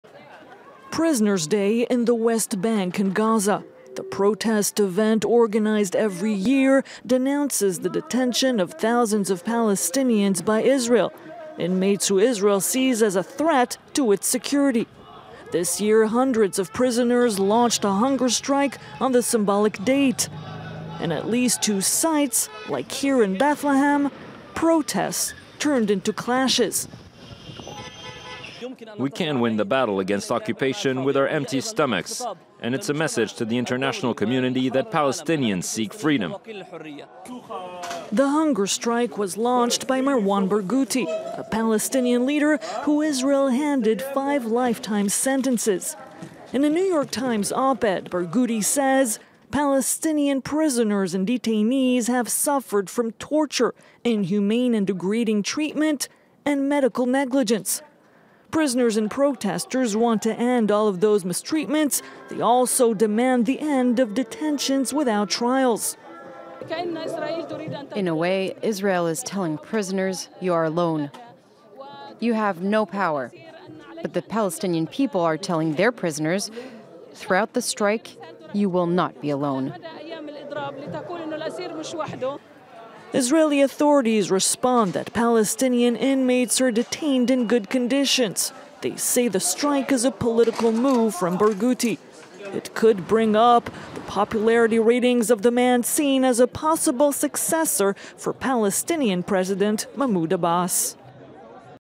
Israel: Palestinian inmates launch mass hunger strike in jails
Hundreds of Palestinians in Israeli jails launched a hunger strike on Monday following a call from leader and prominent prisoner Marwan Barghouti, a Palestinian Authority official said.